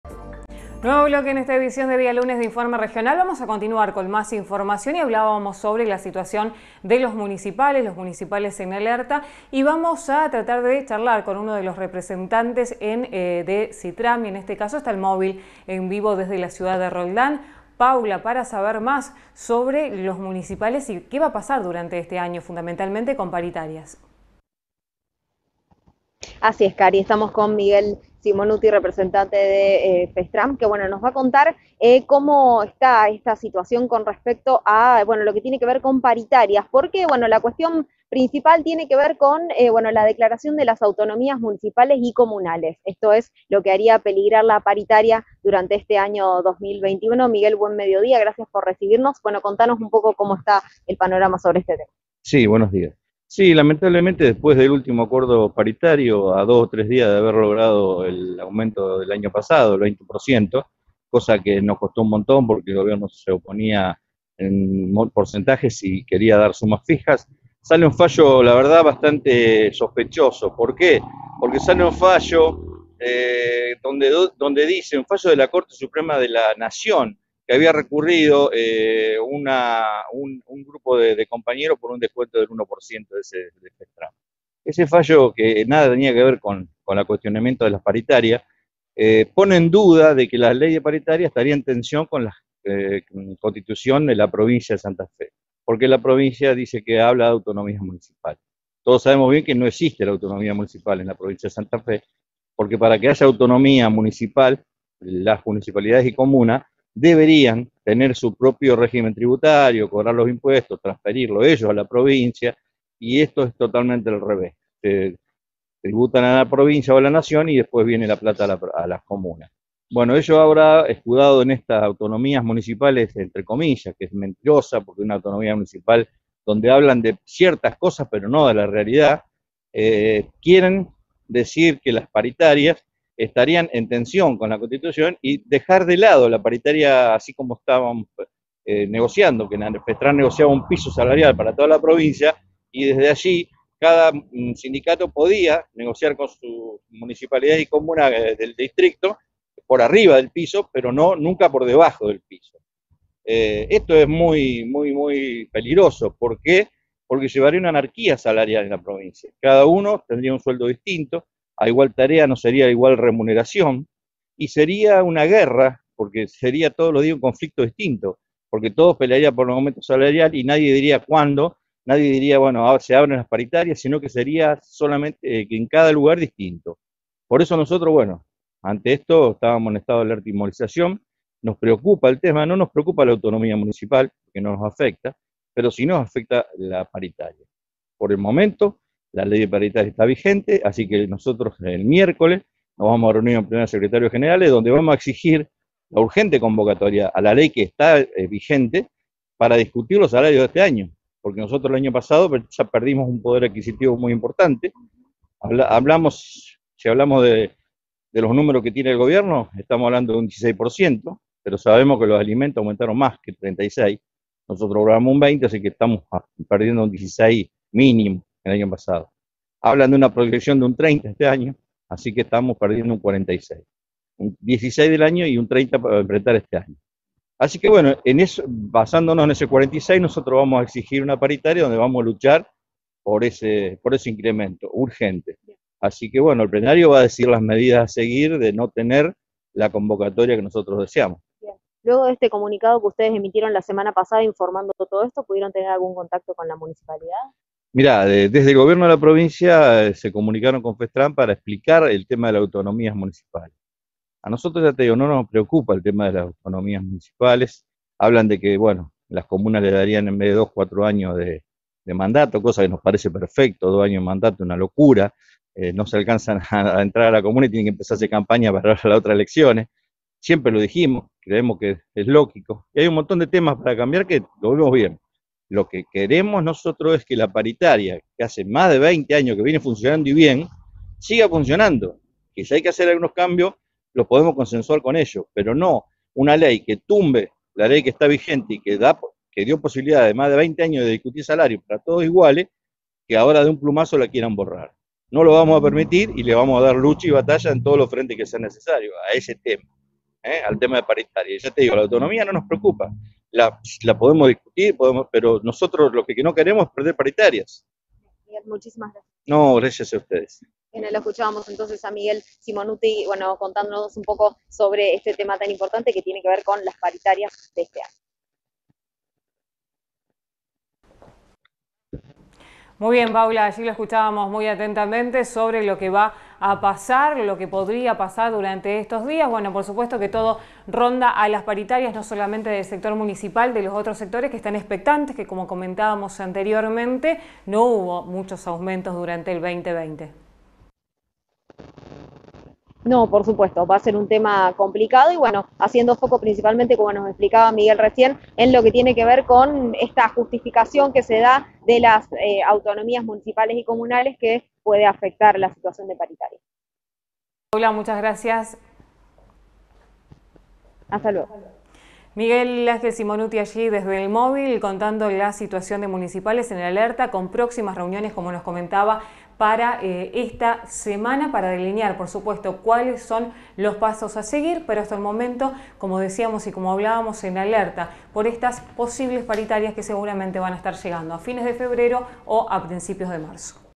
Municipales santafesinos aguardan por las paritarias
Los trabajadores municipales de Santa Fe se encuentran en estado de alerta ante las inminentes paritarias que avisoran un nuevo conflicto con las autoridades estatales.